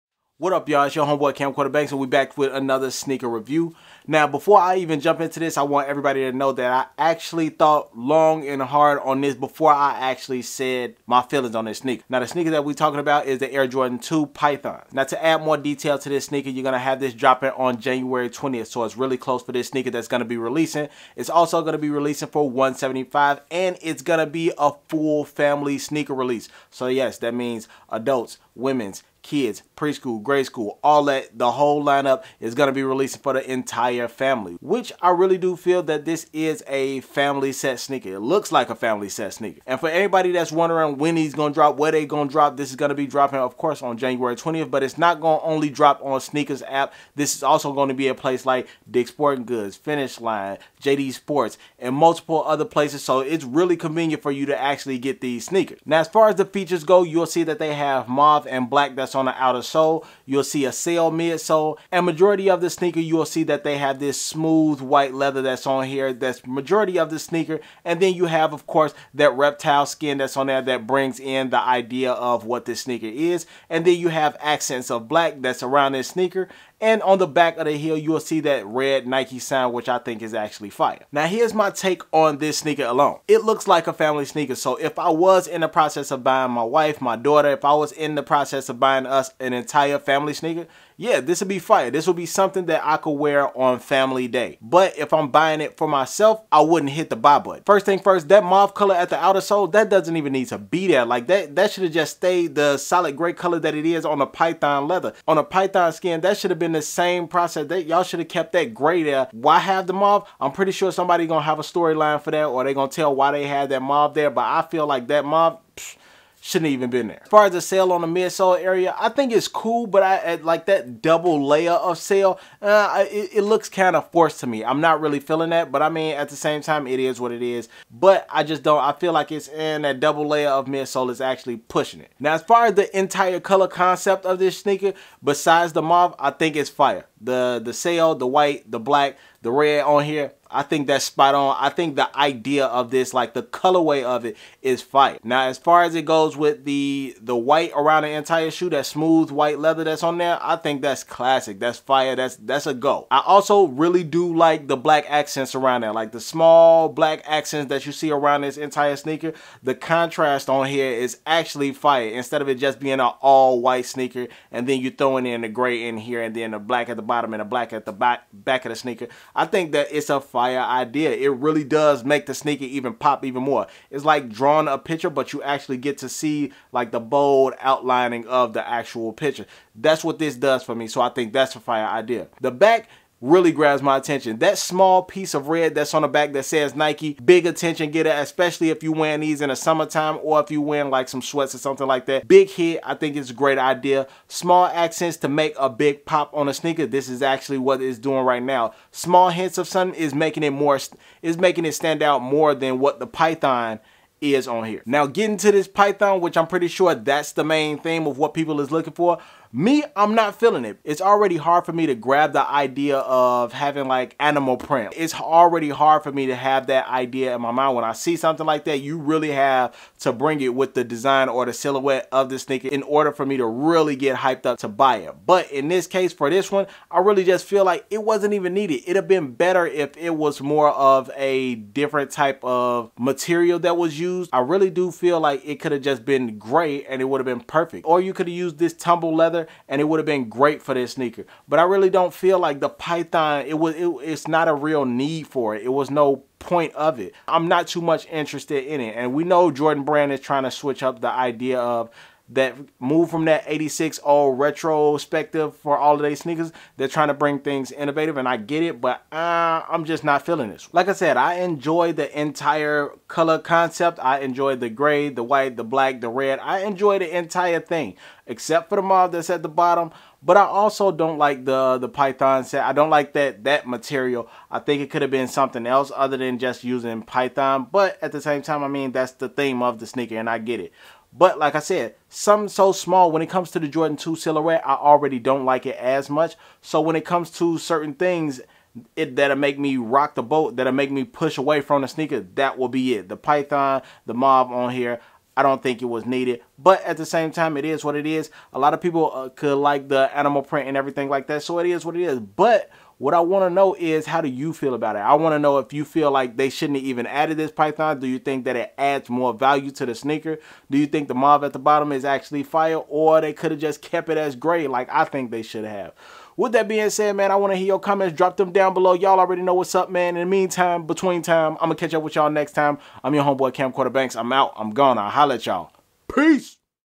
What The Game Been Missing? | Jordan 2 "PYTHON" | Sneaker Review
The sneaker game continues to change year after year. But is all change good? Can this innovative Air Jordan 2 " Python " design, be the creation the the game have been missing?

See how I feel about the new coming release. Check out when the release date will be, who is this sneaker specifically for, and check out the price.

Could this pull us out of the creative drought? Do Air Jordans only sell when there is a collaboration?

Let's see.